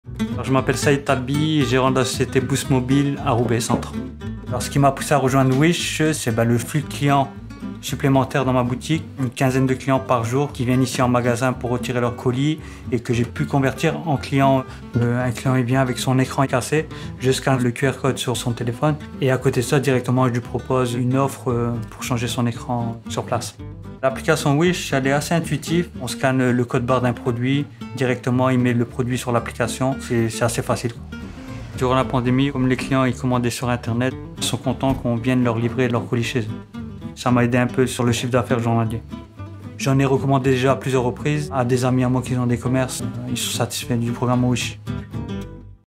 Boost Mobile's Wish Local Partnership
Supply your store with a steady stream of customers by partnering with Wish Local! Check this out for details on how to partner with us.

Meanwhile, Said Talbi, owner of Boost Mobile in France, is here to tell you all about his experience as part of the Wish Local program.